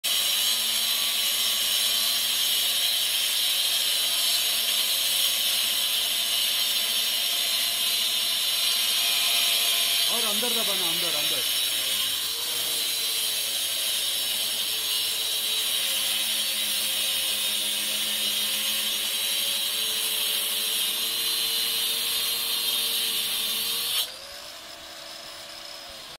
Dust-free and precise groove cutting on concrete & bricks | Wall Chaser SG1251J
A 1 400 W wall chaser with constant electronics that cuts precise grooves in concrete and bricks without generating dust. Two diamond wheels in operation simultaneously. Max. cutting depth of 30 mm, with an adjustable width of 6 – 30 mm. Diamond wheels included.